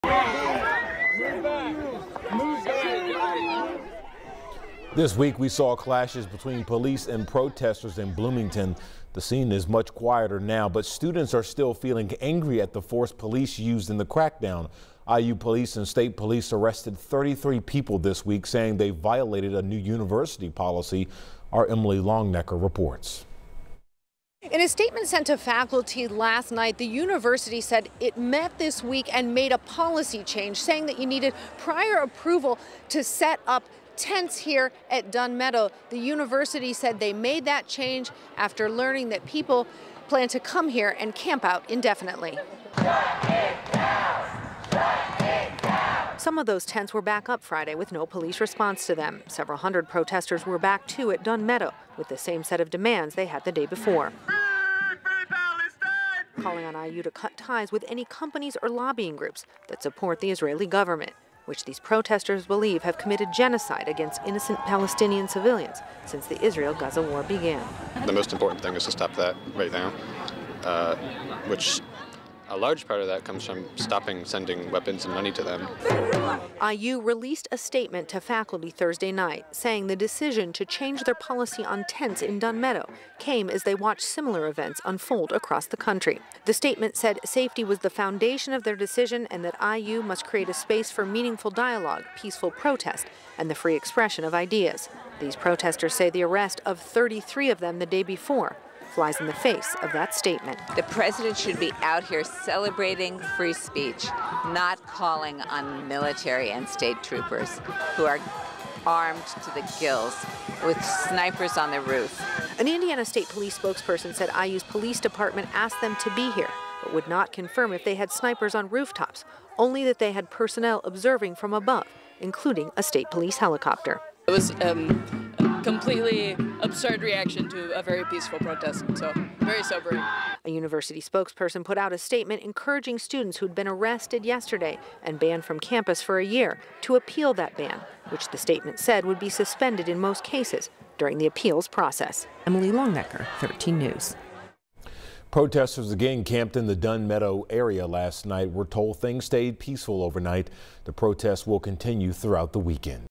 Indiana University Bloomington police and protestors clash during protests, 33 arrested
After the protests, students are angry at the force police used in the crackdown.